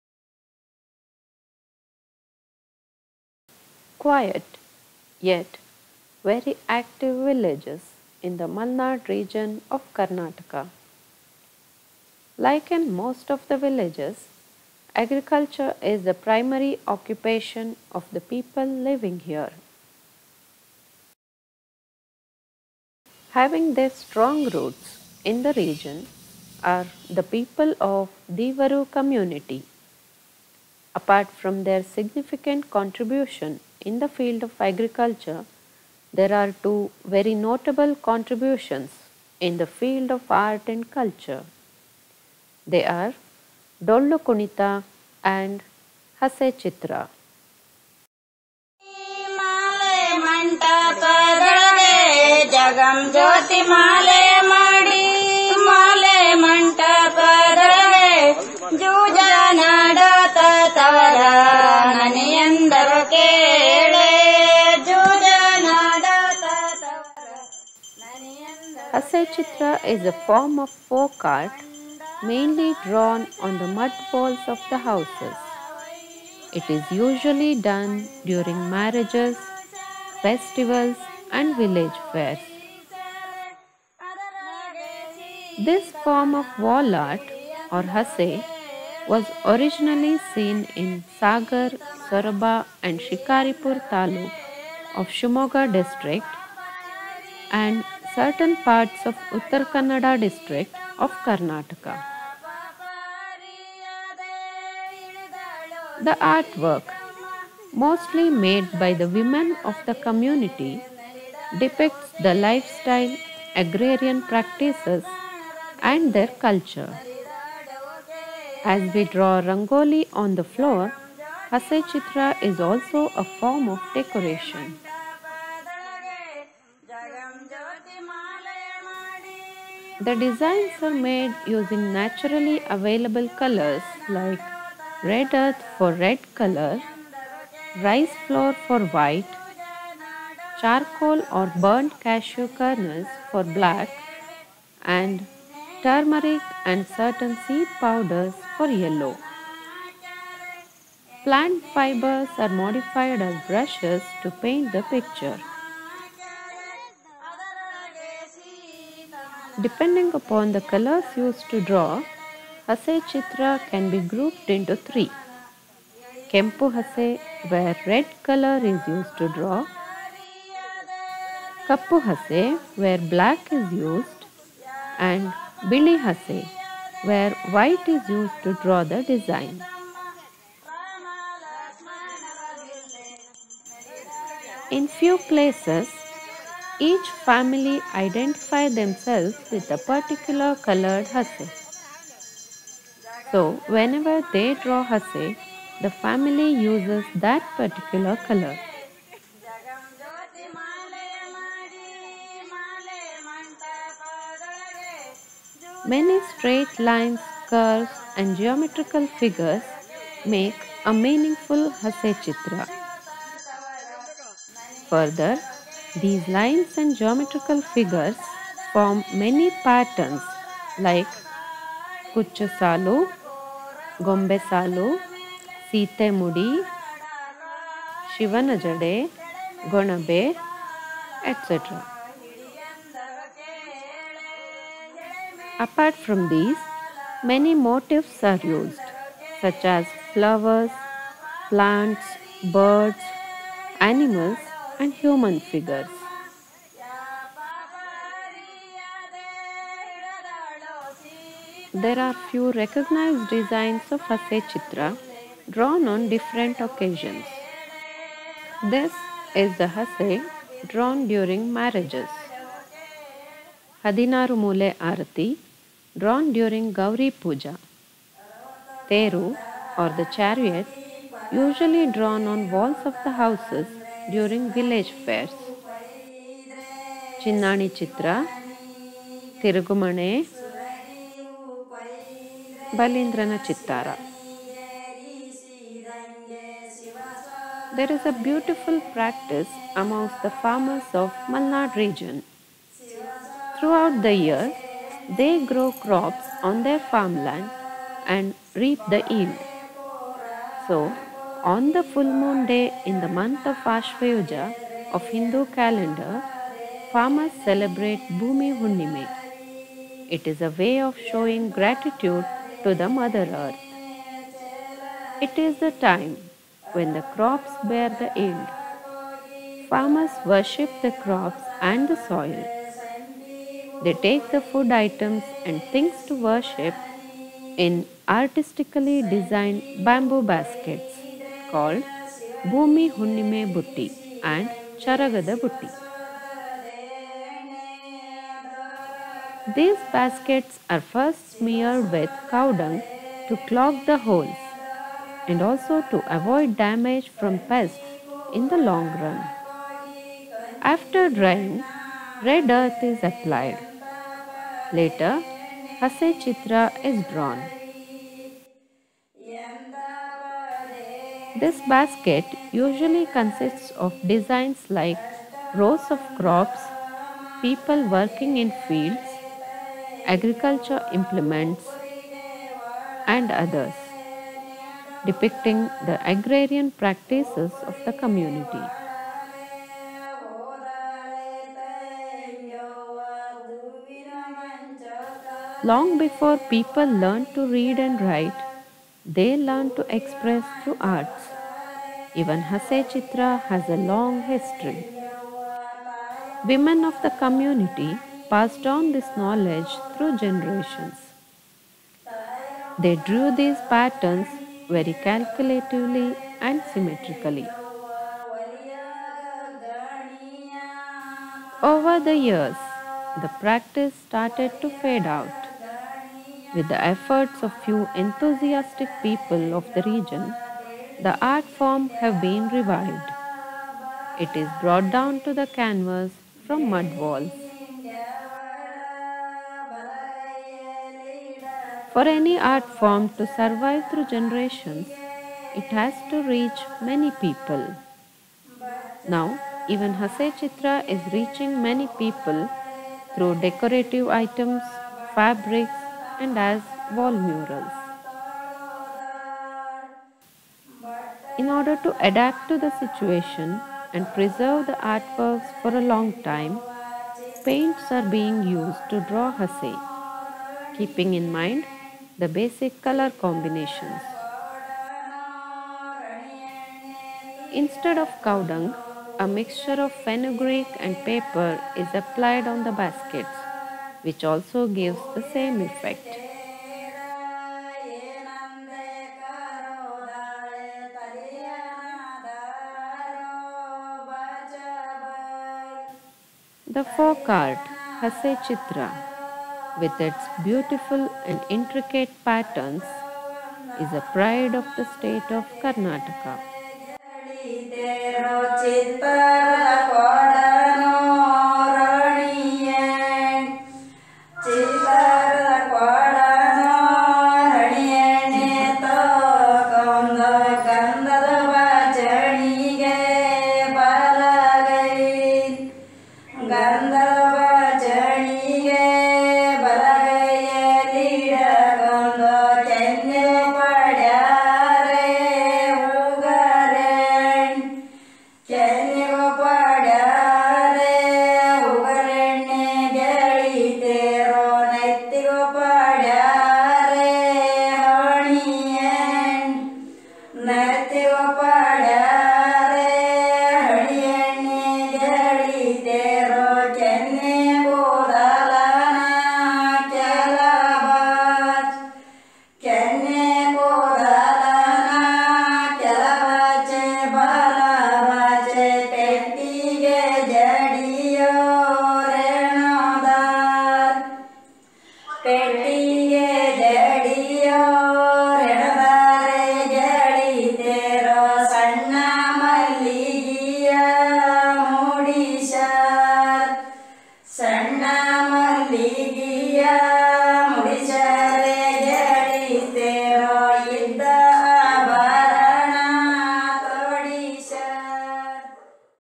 ಹಸೆ ಚಿತ್ರ - ಕರ್ನಾಟಕದ ಜಾನಪದ ಕಲೆ/Hase Chitra - Folk art of Karnataka/Documentary on Folk art of India
Folk art is a beautiful medium of expression adapted by the earlier civilization.
Now it is a good source to information about the earlier people of a particular region, their lifestyle and practice, culture etc.
Folk art maybe visual art or performing art. Every folk art is unique in nature and carries its own values.

Hase Chitra originated in Malnad region of Karnataka, India; practiced mainly by the Deevaru community.